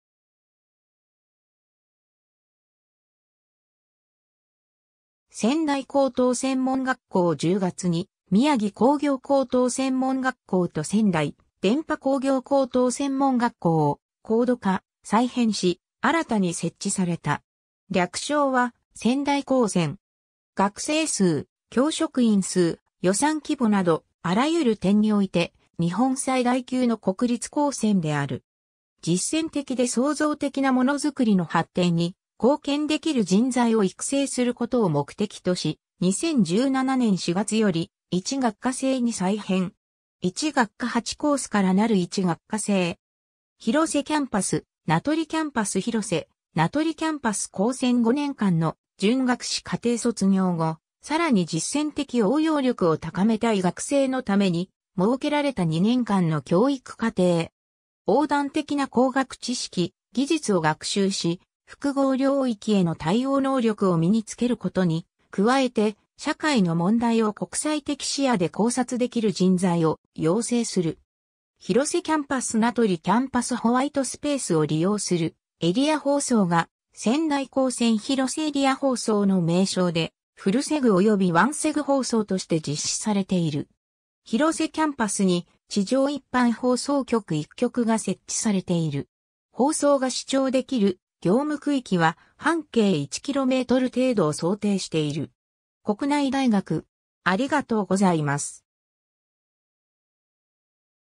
仙台高等専門学校
仙台高等専門学校（せんだいこうとうせんもんがっこう、英称：National Institute of Technology (KOSEN), Sendai Collegeは、宮城県仙台市青葉区愛子及び名取市愛島にキャンパスを置く日本の国立高等専門学校である。 2009年（平成21年）10月に宮城工業高等専門学校（宮城高専）と仙台電波工業高等専門学校（仙台電波高専）を高度化・再編し、新たに設置された。
略称は「仙台高専」。
 学生数、教職員数、予算規模など、あらゆる点において日本最大級の国立高専である。
 実践的で創造的なものづくりの発展に貢献できる人材を育成することを目的とし，2017年（平成29年）4月より1学科制に再編。
1学科8コースからなる1学科制。
 広瀬キャンパス 名取キャンパス　 広瀬・名取キャンパス 高専5年間の準学士課程卒業後、さらに実践的応用力を高めたい学生のために設けられた2年間の教育課程。
 横断的な工学知識・技術を学習し複合領域への対応能力を身に付けることに加えて、社会の問題を国際的視野で考察できる人材を養成する。
 広瀬キャンパス 名取キャンパス ホワイトスペースを利用するエリア放送が、「仙台高専広瀬エリア放送」の名称でフルセグおよびワンセグ放送として実施されている。
 広瀬キャンパスに地上一般放送局1局が設置されている。
 放送が視聴できる業務区域は半径1km程度を想定している。
 国内大学